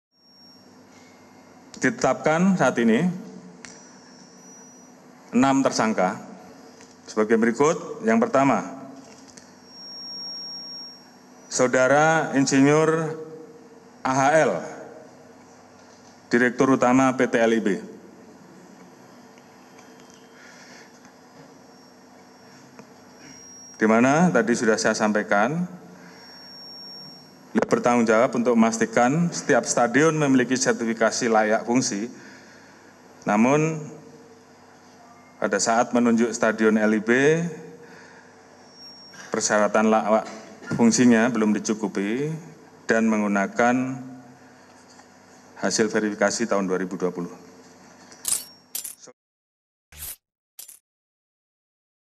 POLRI TETAPKAN DIRUT LIGA INDONESIA BARU SEBAGAI TERSANGKA TRAGEDI KANJURUHAN
Kapolri mengumumkan bahwa Direktur Utama Liga Indonesia Baru, Akhmad Hadian Lukita sebagai salah satu tersangka Tragedi Stadion Kanjuruhan yang terjadi pada laga Arema vs Persebaya, 1 Oktober 2022 yang lalu. AHL dijadikan tersangka karena tidak melengkapi persyaratan fungsi dan menggunakan hasil verifikasi stadion tahun 2020.